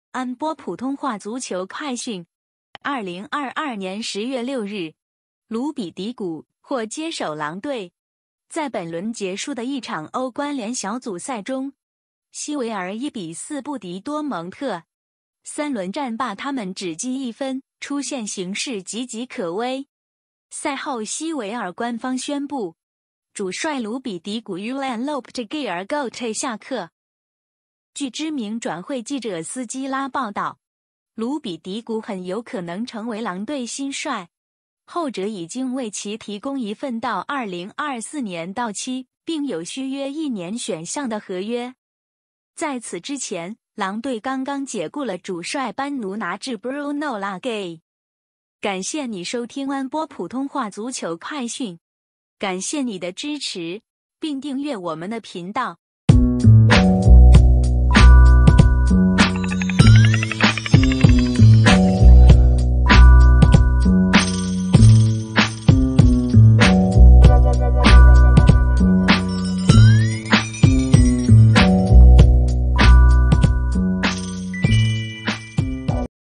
(普)【歐冠-最新消息】2022-10-06 盧比迪古或接手狼隊
在本輪結束的一場#歐冠 聯小組賽中，西維爾 1比4不敵#多蒙特，三輪戰罷他們只積1分，出線形勢岌岌可危。

賽後西維爾官方宣佈，主帥#盧比迪古 (Julen Lopetegui Argote)下課。

據知名轉會記者斯基拉報導，盧比迪古很有可能成為狼隊新帥，後者已經為其提供一份到2024年到期，並有續約一年選項的合約。在此之前，狼隊剛剛解僱了主帥般奴拿治(Bruno Lage)。